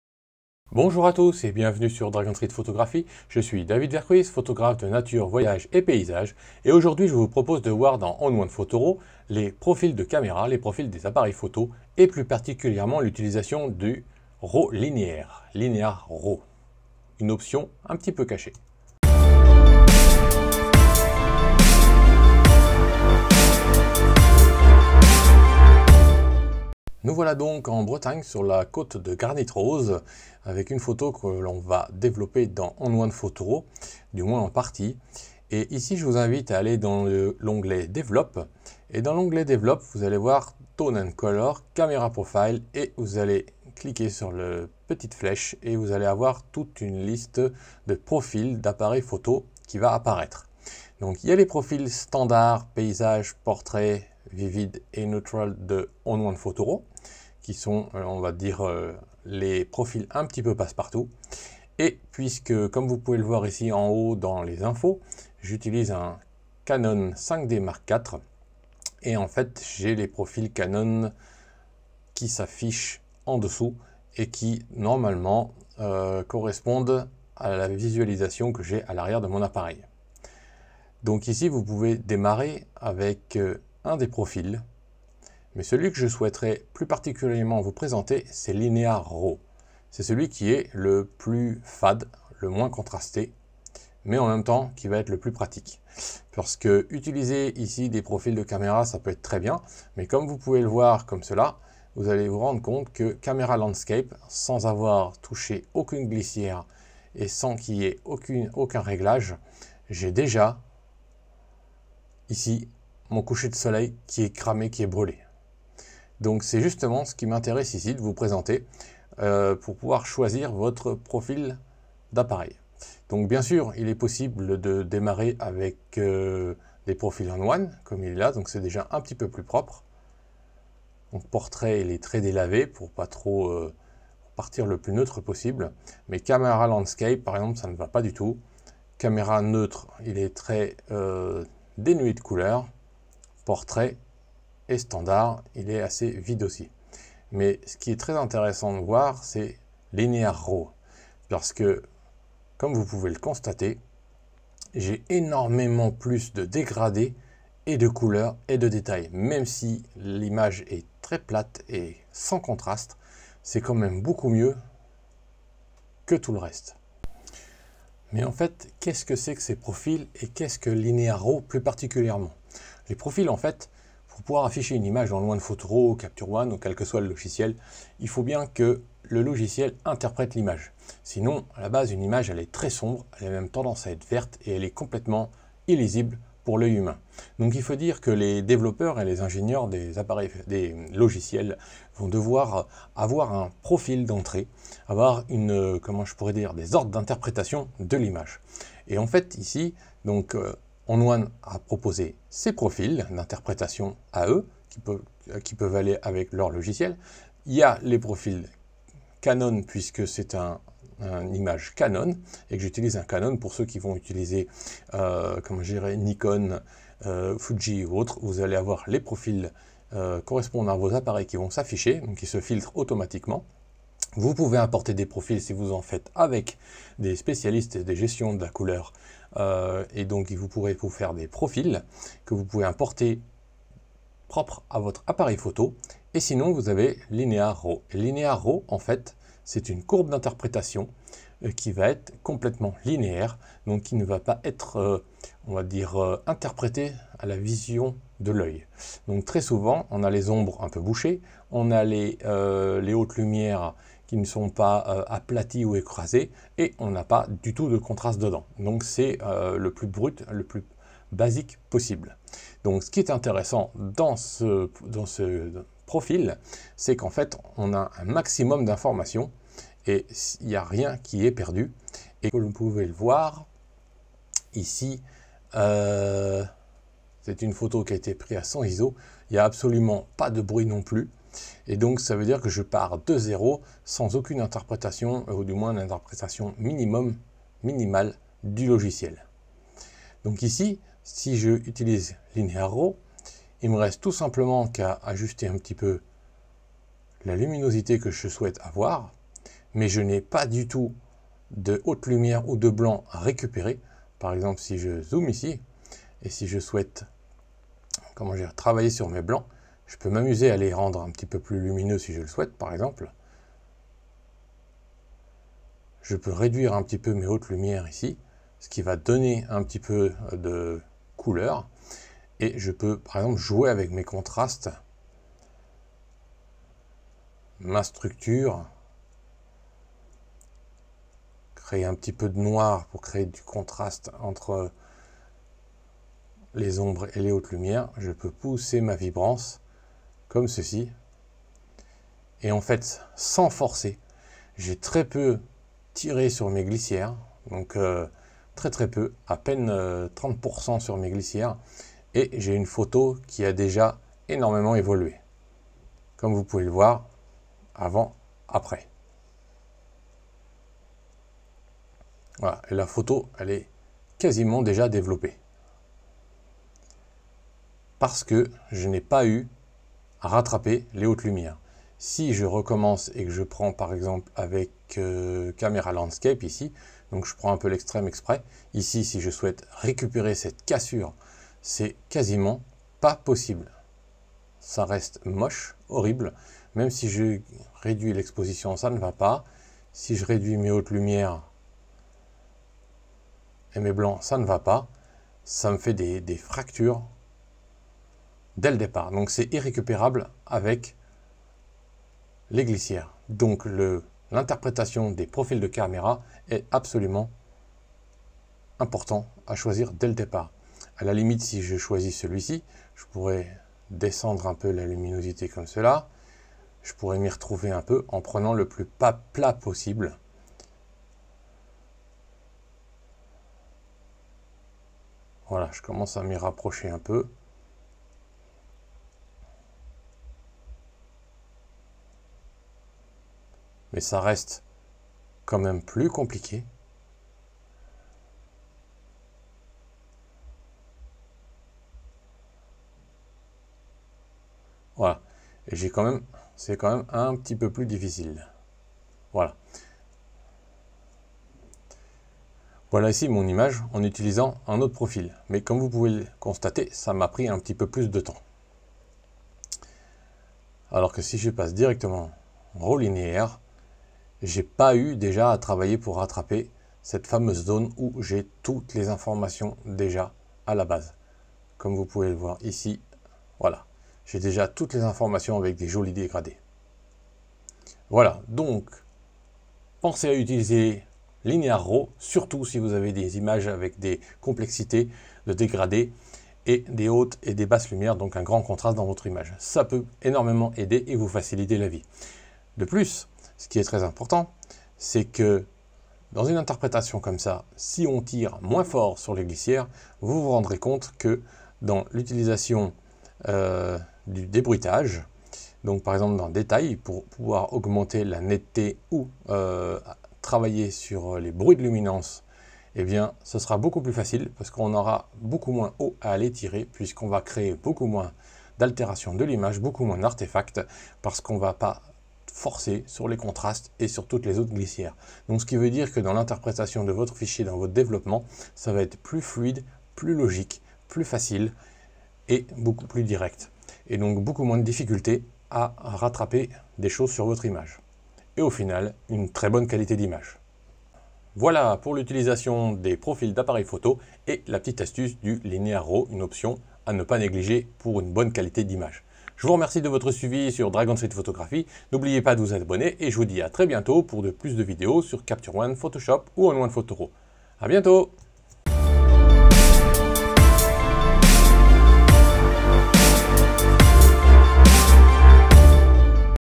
Camera Profil et l'option Linear Raw
Que sont les profils d’appareil «  Camera Profil » et pourquoi utiliser le profil « Linear Raw » d’On1 Photo Raw.

Si vous souhaitez tester On1 Photo RAW 2021 pendant 30 jours

Obtenez 15% sur les produits On1 Photo RAW avec le code ” DRAGONSTREET "